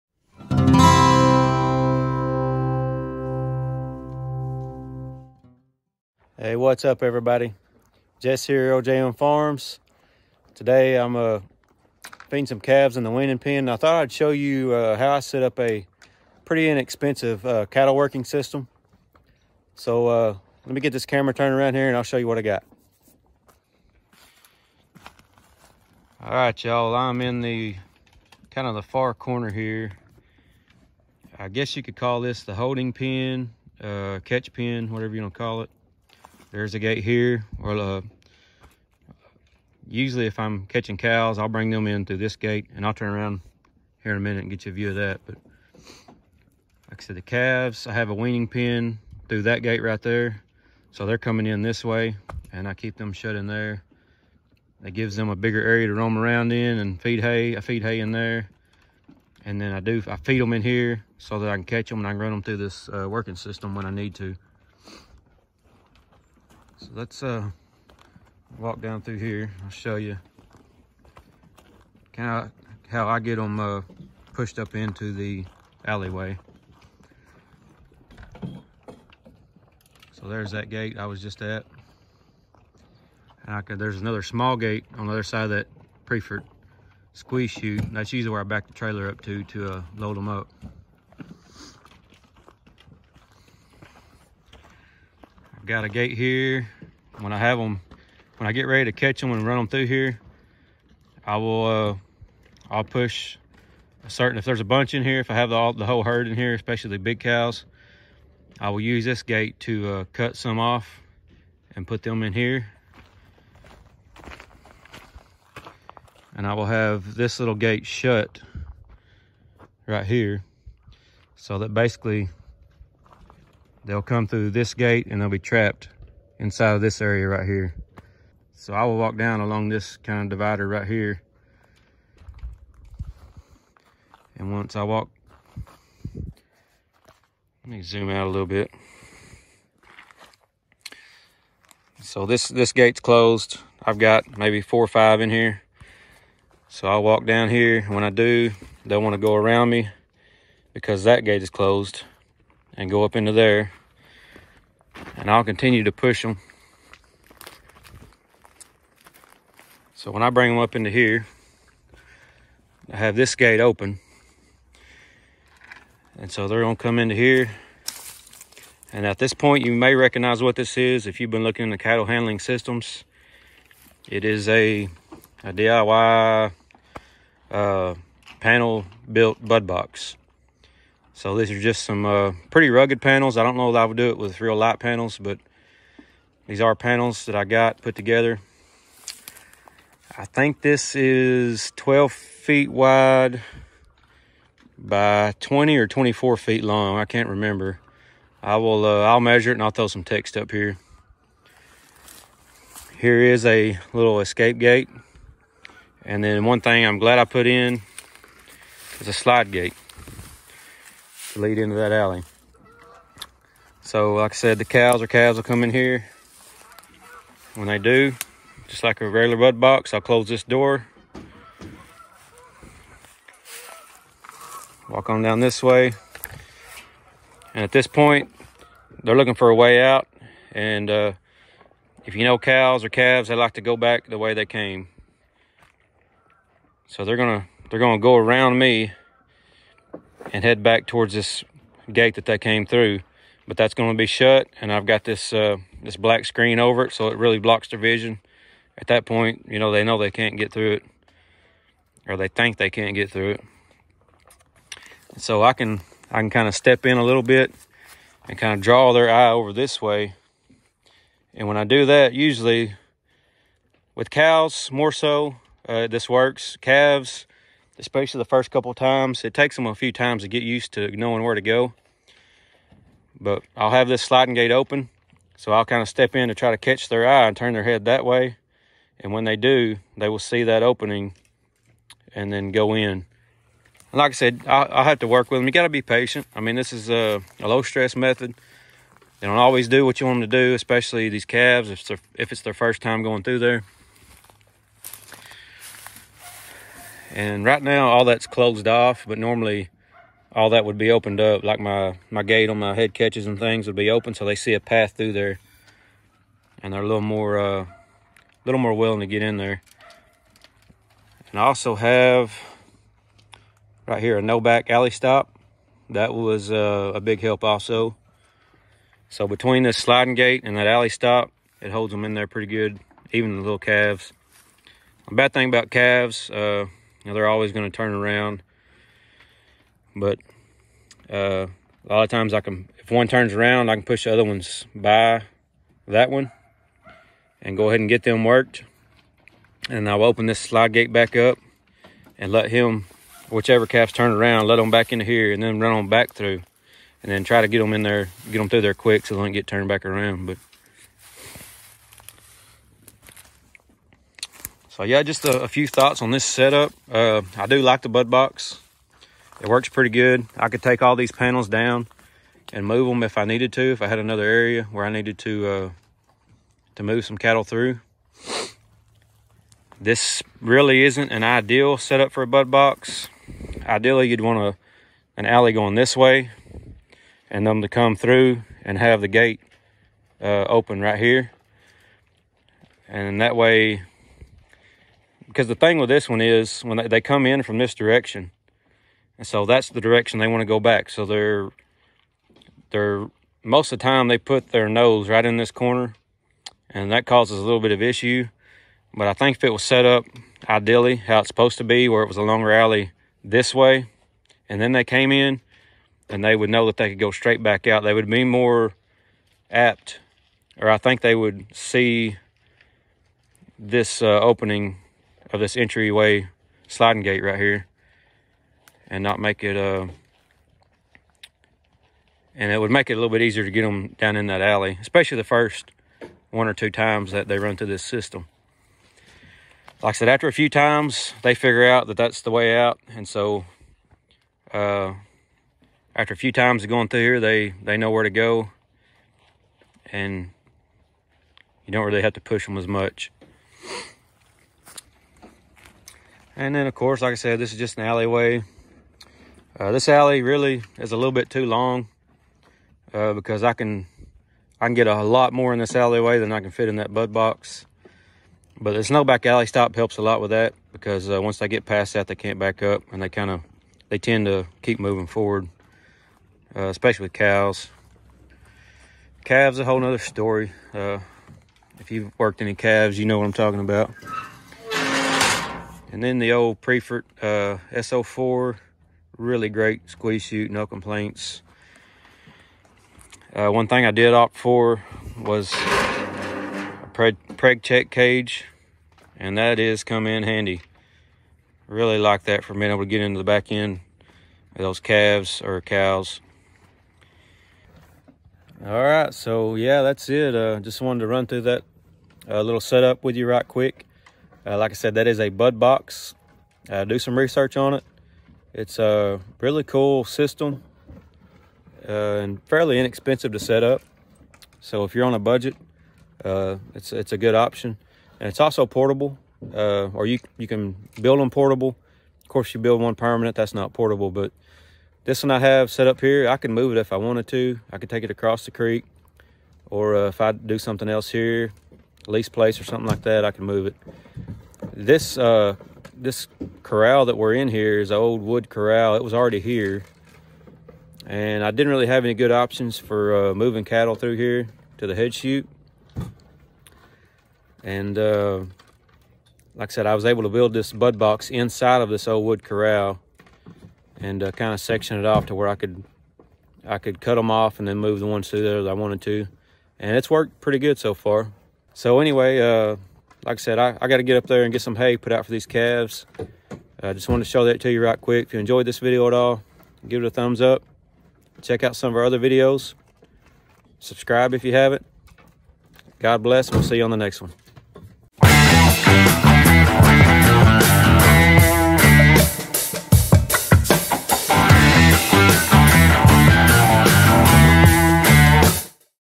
Low Cost - Low Stress - Cattle Handling System - Walkthrough
Check out Temple Grandin Books for Livestock Handling Designs! I have the Kindle version.

How I constructed a Low Cost, Low Stress, cattle handling system inside of an older wood corral. The corral has seen better days, but I was able to construct a bud box inside using portable panels. This system allows me to run the cattle through using low stress methods and has really made working the cattle more enjoyable since the stress level on both us and the cattle are lower!